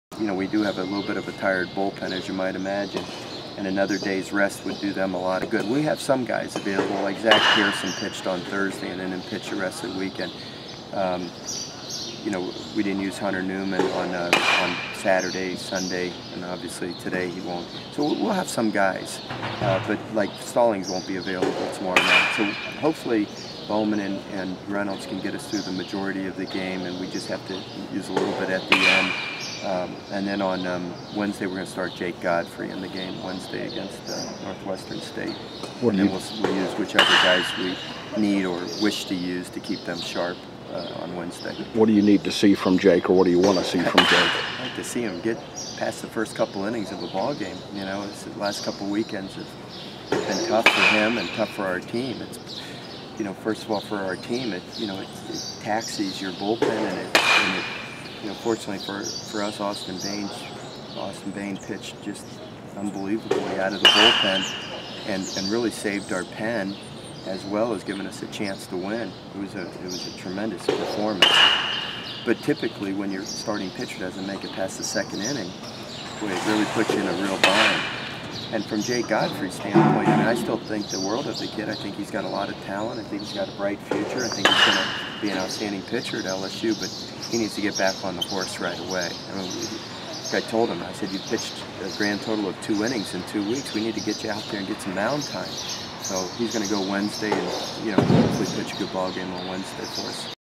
LSU will lean on veterans Kyle Bouman, Russell Reynolds vs. UNO and then Jake Godfrey on Wednesday
Tigers coach Paul Mainieri wants to give bullpen as much rest as possible.